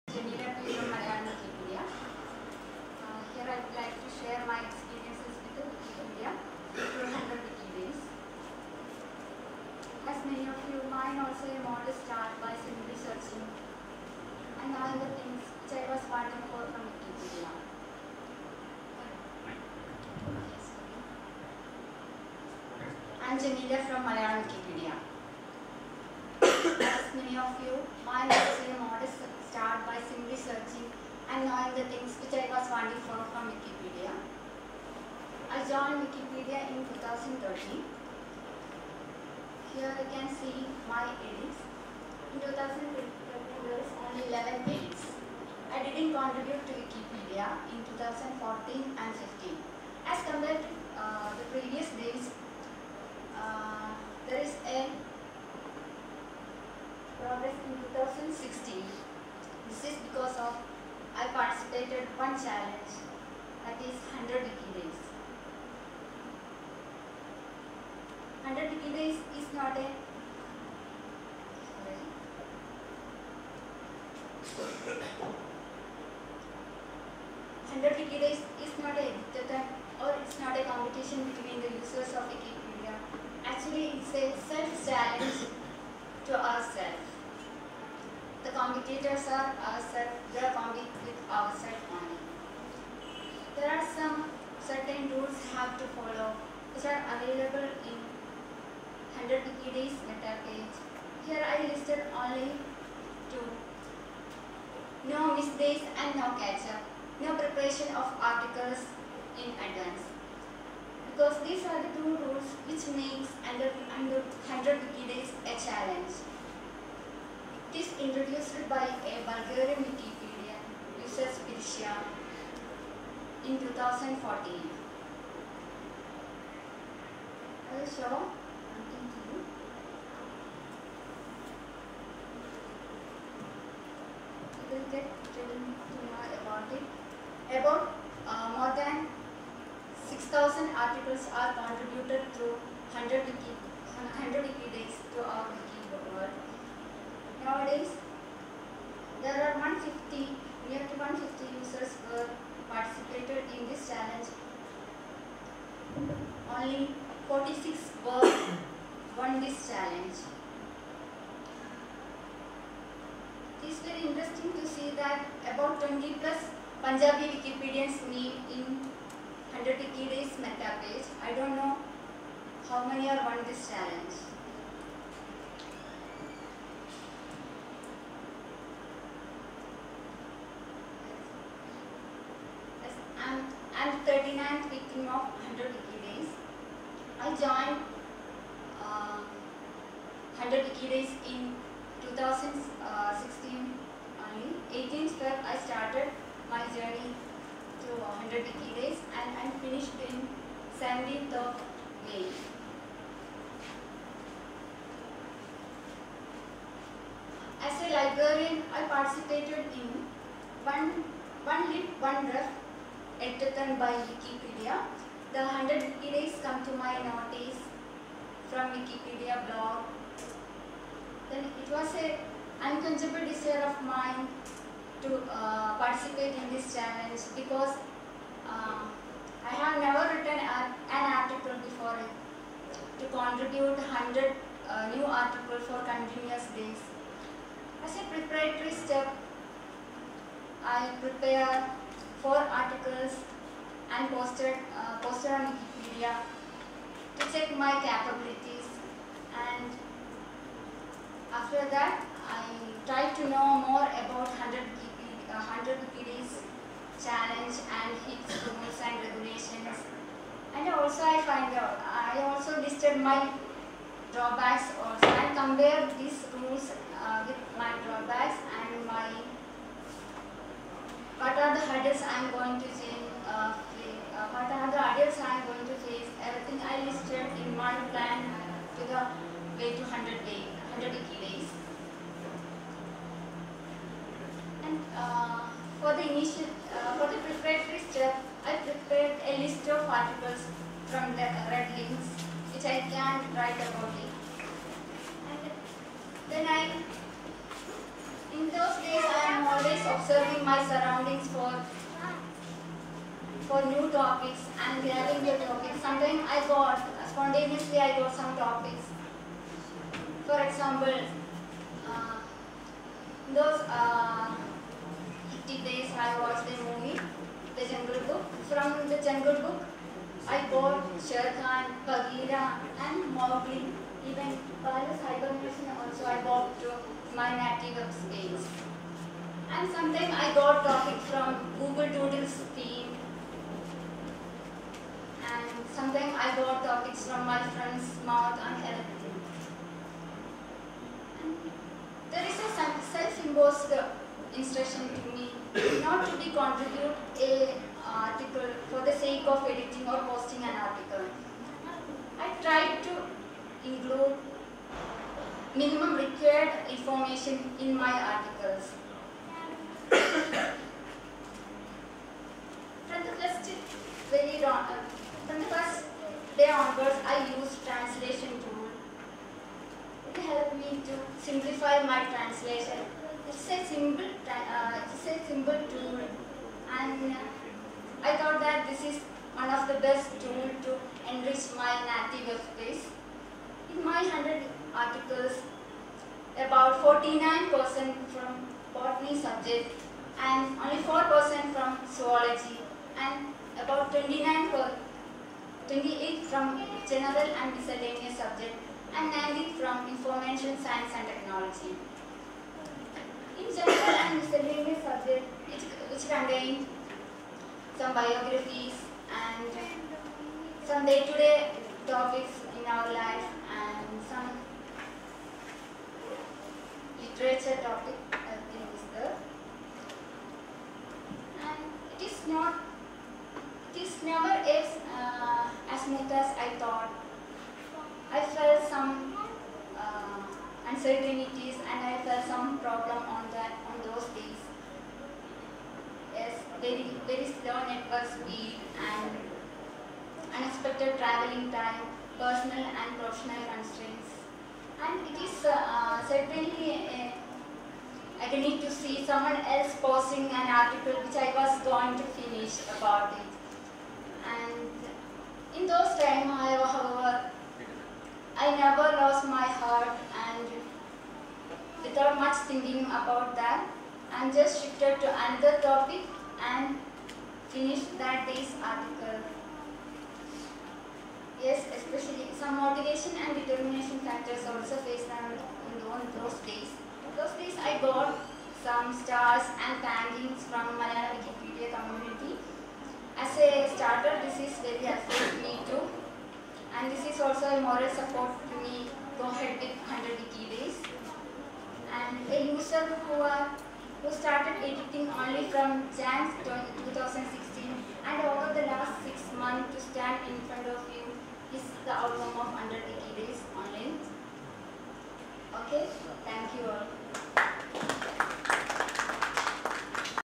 My wiki journey – mere 11 to 1100 and on- experience of 100wikidays at WCI2016
My wiki journey – mere 11 to 1100 and on- experience of 100wikidays at WikiConference India 2016 by Jameela P.